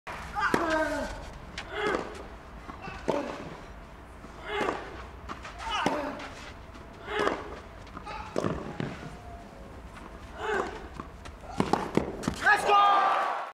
Hot Shot: 'Let's Go!' Zverev Fires Stunning Backhand Passing Shot In Munich 2019
Watch Hot Shot as Alexander Zverev finds the target with a backhand passing shot on the run to earn two set points against Juan Ignacio Londero at the BMW Open by FWU. Photo credit: Alexander Hassenstein/Getty Images for BMW. Video courtesy of BMW Open by FWU.